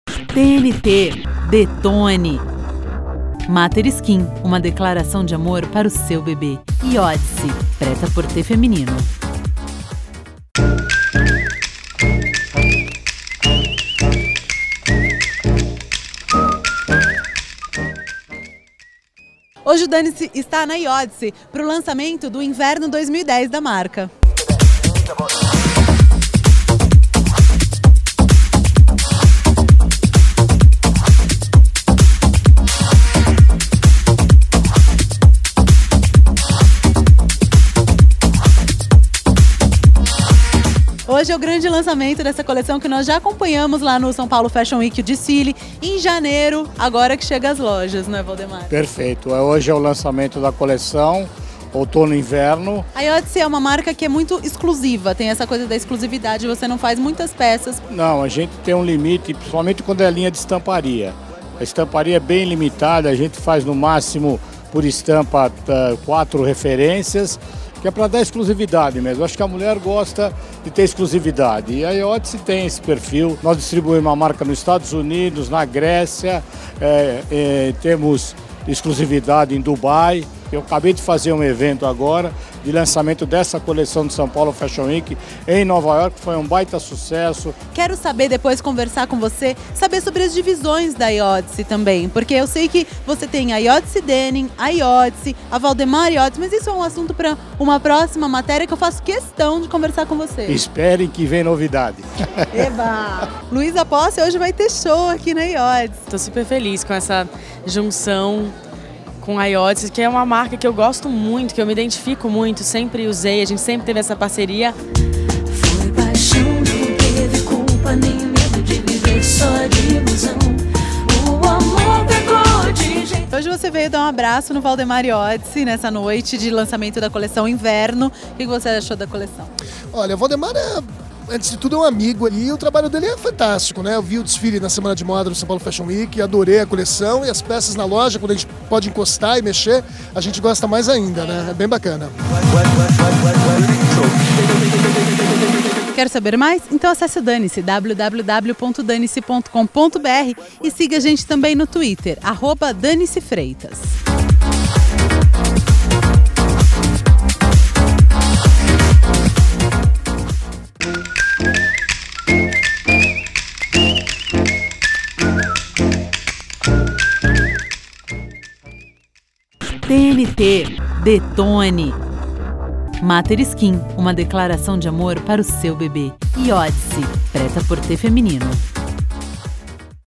Iódice/Coleção Inverno 2010
O Dani-Se mostra o lançamento da coleção outono/inverno 2010 da Iódice que aconteceu na Flagship da marca na rua Oscar Freire, muita gente estava por lá conferindo a nova coleção que é inspirada na Amazônia.
Fauna e flora inspiraram estampas, cores e texturas, o preto que é uma das características da mulher Iódice vem menos sexy e mais elegante, os fortes também são os vestidos nude com wet leggings, meias-calças de renda ou bordadas com paetês.
Se você não conseguiu ver o desfile ou as peças da coleção, não sabe o que está perdendo!